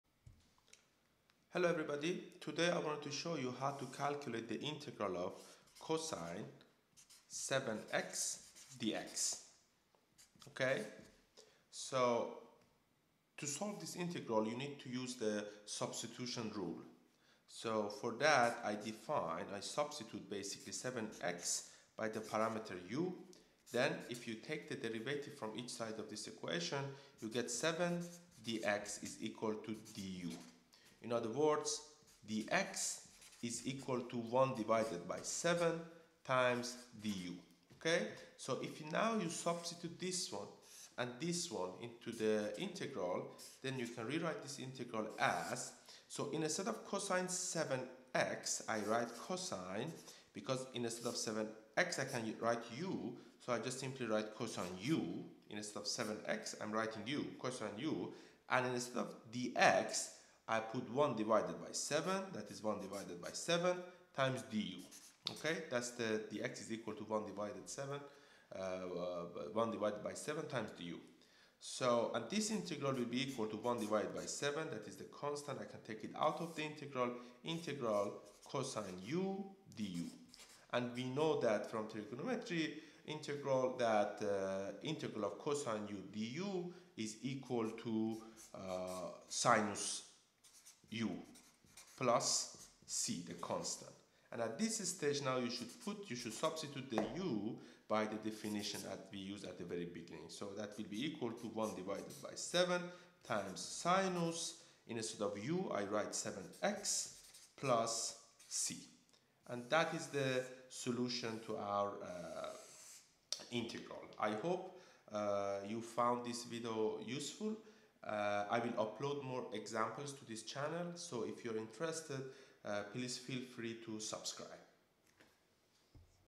Integral of cos(7x) - Integral example
This video shows how to find the integral of cos(7x).